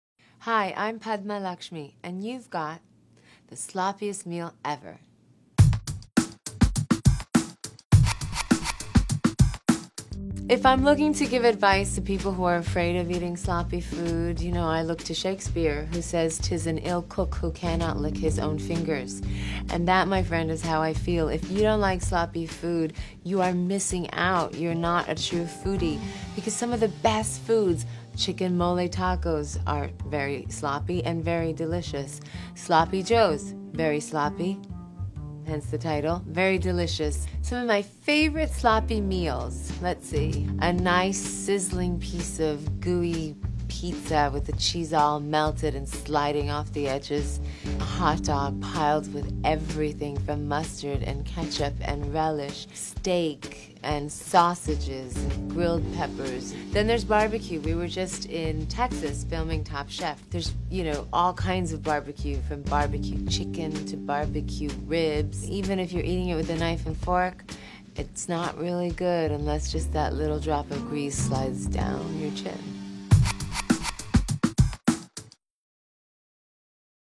You've Got Padma Lakshmi
Top Chef's Padma Lakshmi talks about the most messy, delicious meals she's ever had.